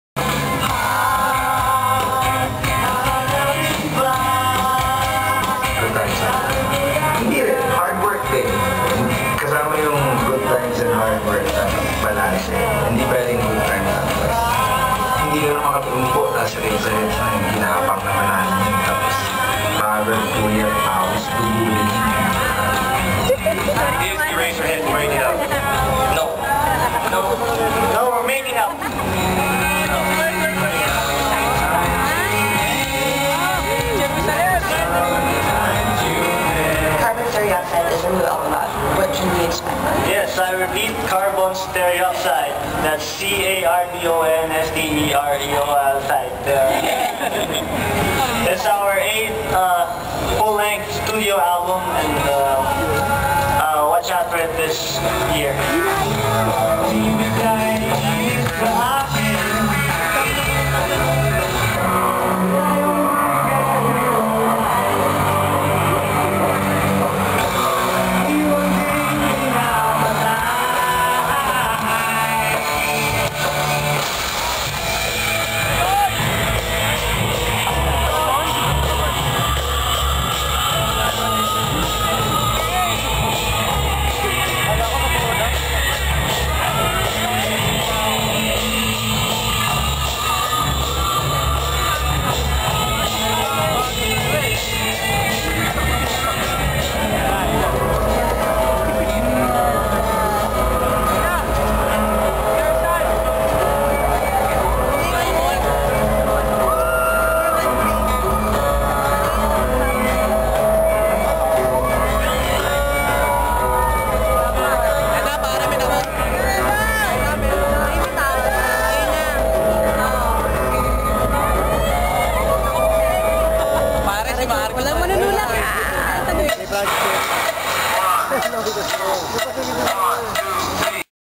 Eheads Final Set Documentary (Before Concert) 2/2
Before concert begins 2 of 2, March 7, 2009.

Sorry, shaky ang vid.